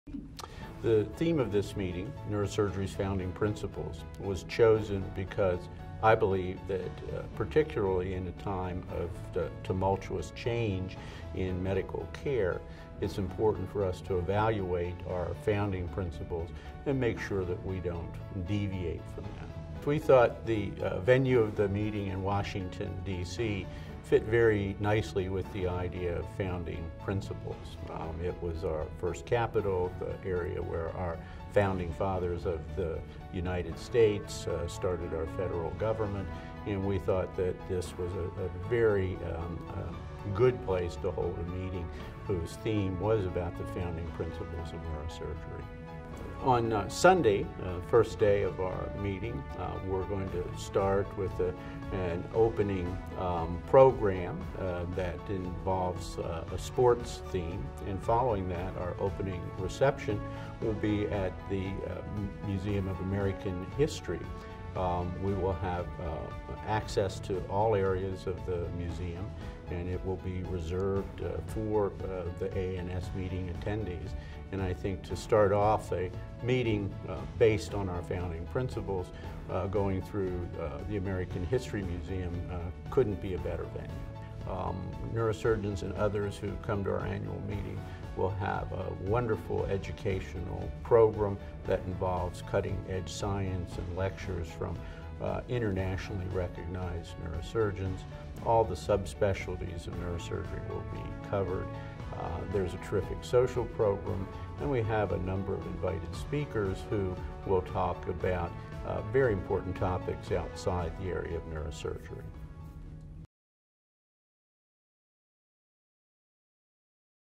Introduction to the 2015 AANS Annual Scientific Meeting
Listen to AANS President Robert E. Harbaugh, MD, FAANS, describe the inspiration behind the 2015 AANS Annual Scientific Meeting's theme, as well as the venues and event location in Washington, D.C.